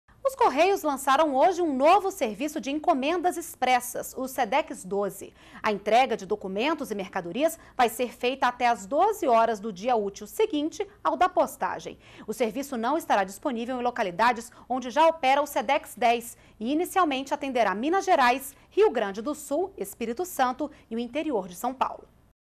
Correios lançaram novo serviço de encomendas expressas
NBR NOTÍCIAS - 02.04.12: Os Correios lançaram, nesta segunda-feira (02), um novo serviço de encomendas expressas, o Sedex 12. A entrega de documentos e mercadorias vai ser feita até as 12 horas do dia útil seguinte ao da postagem. O seviço não estará disponível em localidades onde já opera o Sedex 10 e inicialmente atenderá Minas Gerais, Rio Grande do Sul, Espírito Santo e interior de São Paulo.